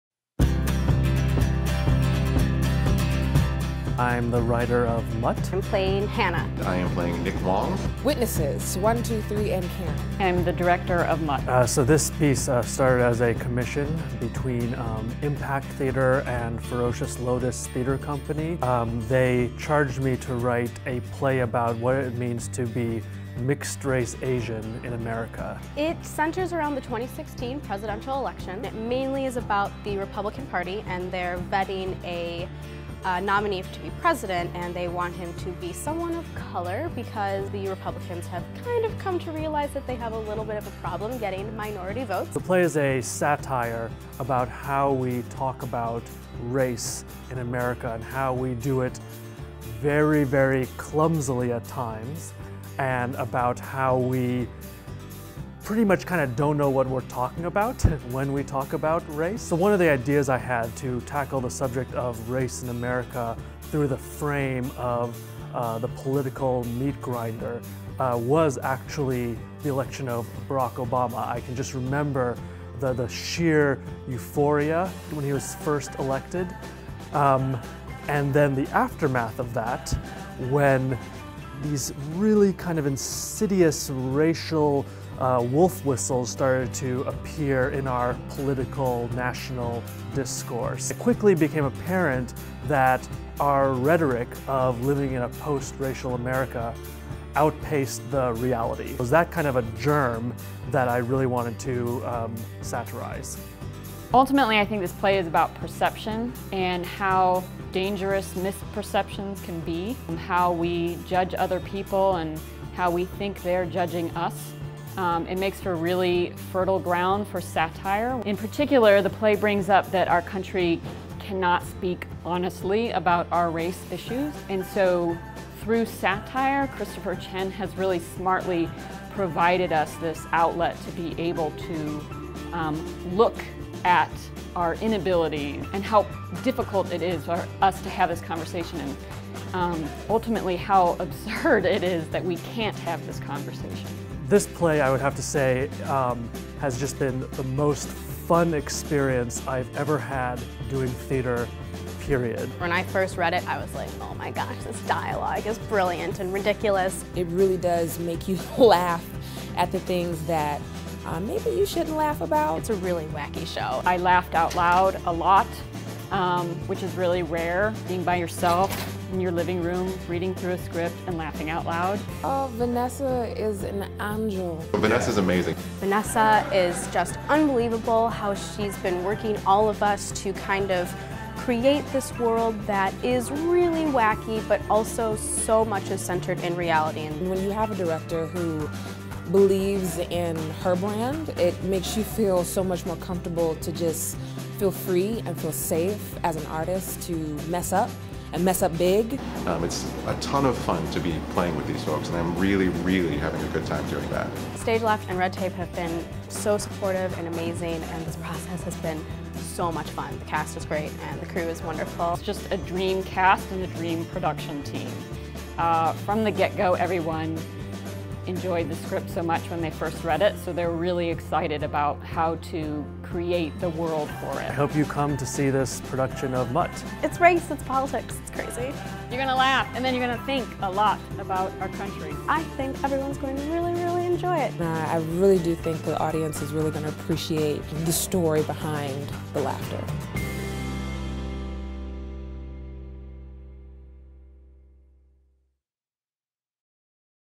MUTT Behind the curtain
Behind the scenes interviews with MUTT playwright Christopher Chen, director Vanessa Stalling and cast members Aurora Adachi-Winter, Nicole Michelle Haskins, and Michael Reyes.

MUTT by Christopher Chen
presented by
Stage Left Theatre and Red Tape Theatre
running 1/9-2/14, 2016
at Theater Wit, 1229 W Belmont in Chicago